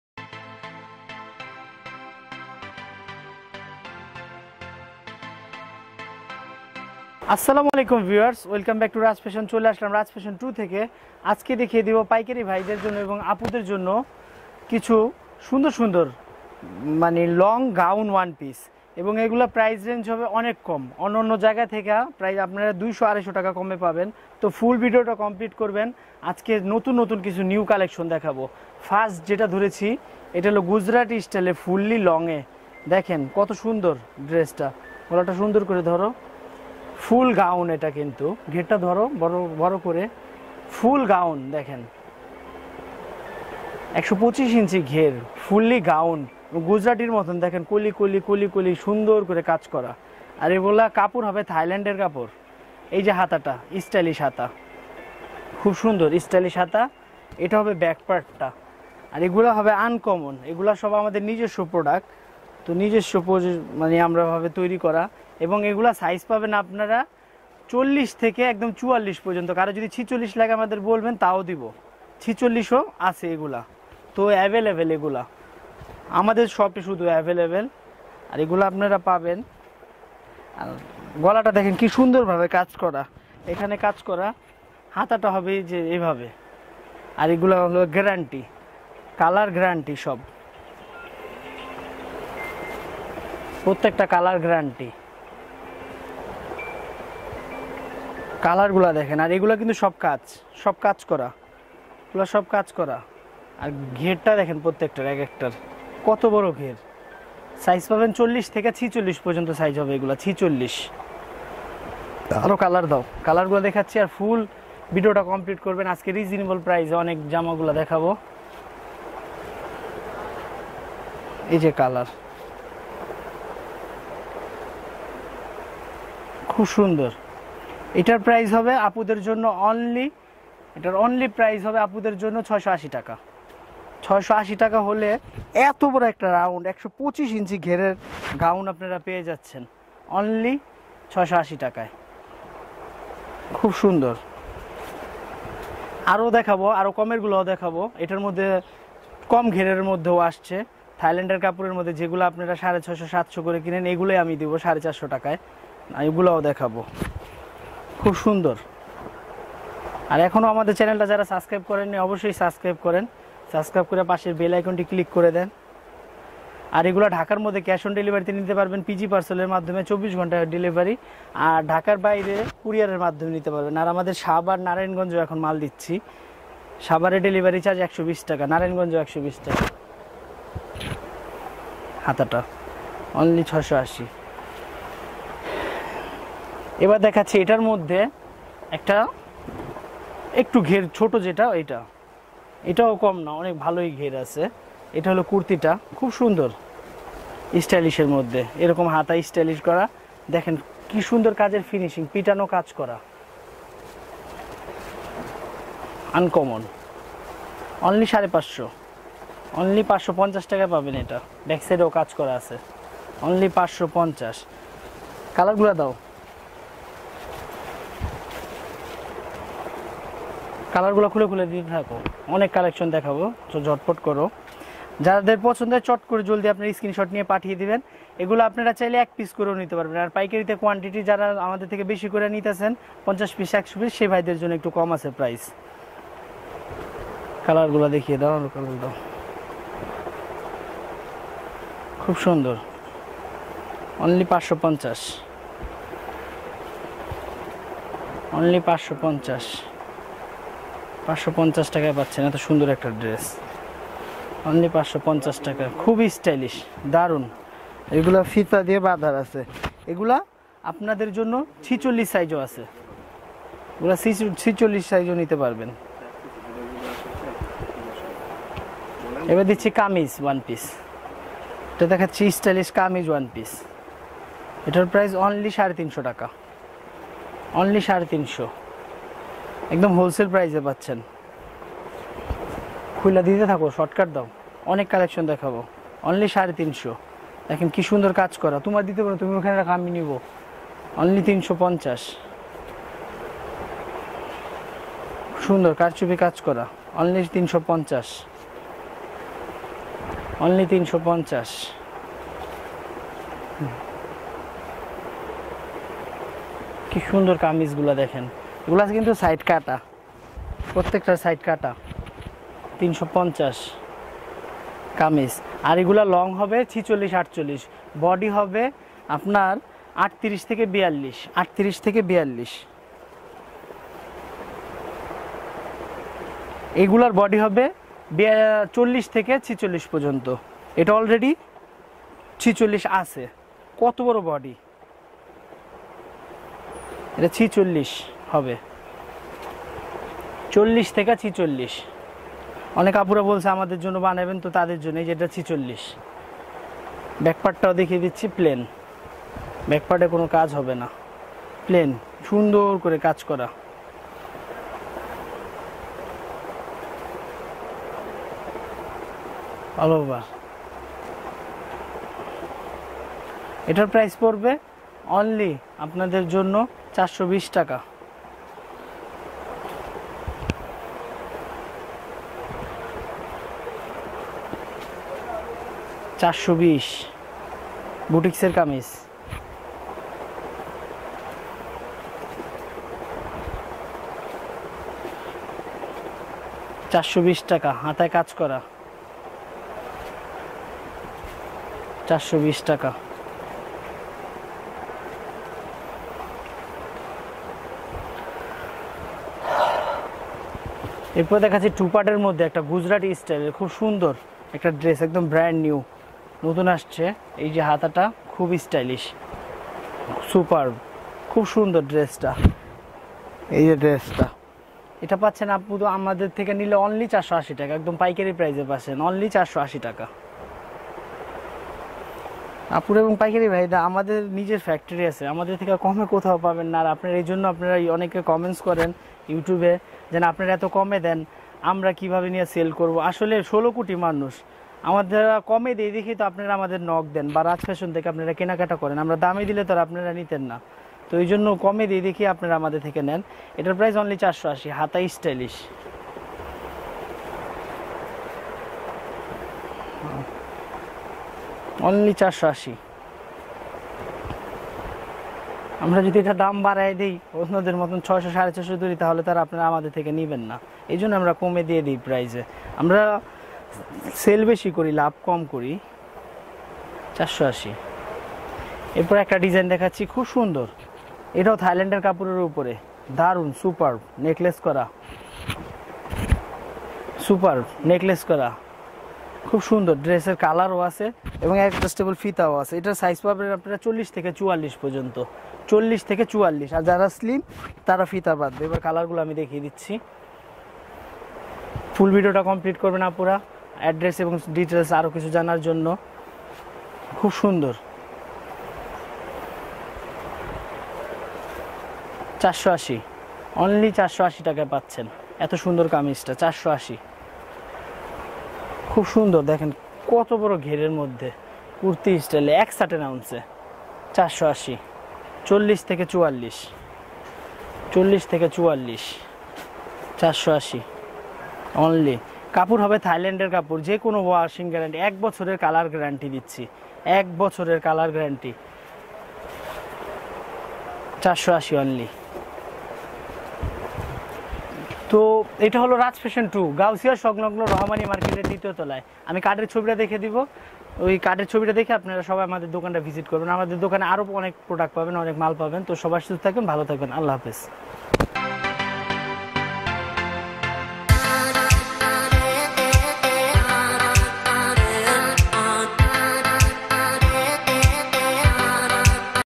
মাথা নষ্ট করার মত কুর্তি কিনুন পাইকারি দামে, গাউছিয়া মার্কেট থেকে
রাজ ফ্যাশন 2